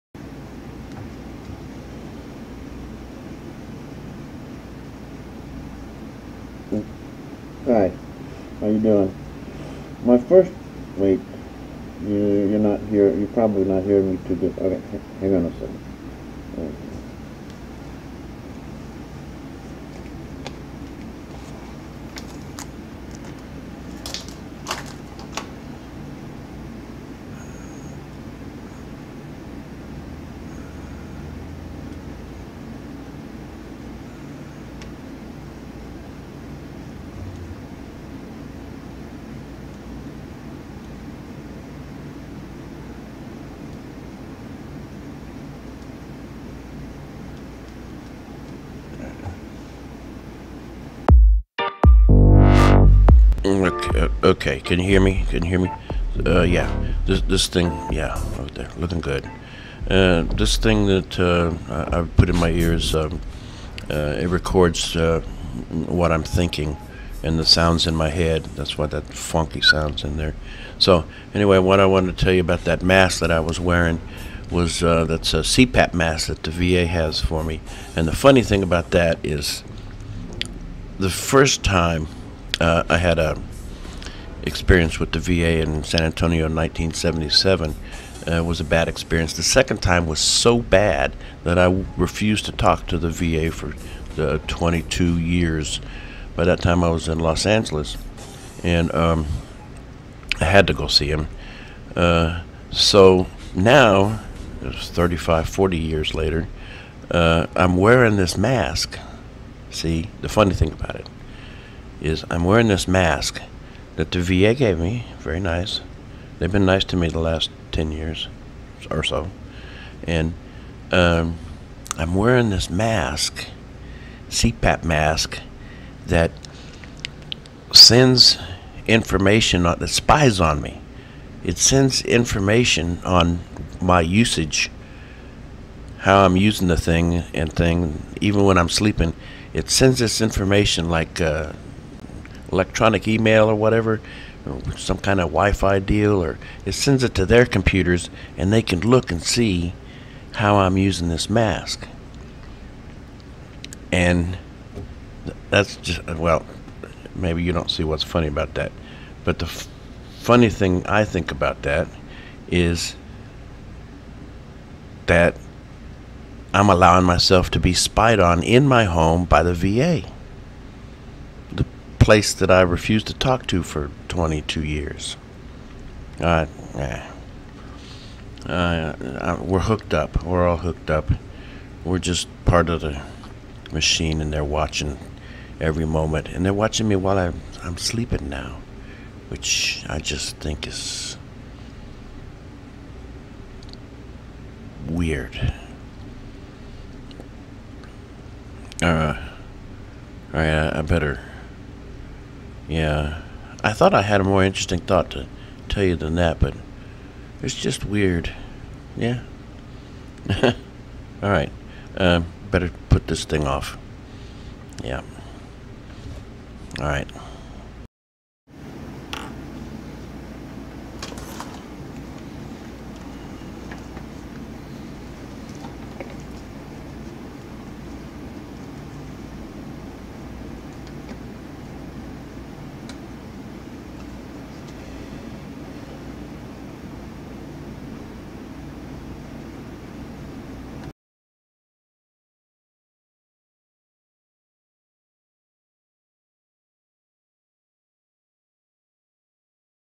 Monitored In My Sleep Nightly
The VA watches me nightly in my sleep and I let them. My younger self would have never allowed this electronic mind-probing. It was kind of creepy the other day when I got a phone call out of the blue from the VA...the techie said something like, "I see that you have been complying with our CPAP usage guidelines so I'm going to send you some replacement hoses and masks." I jumped up and glanced around the room, looking for the spycam.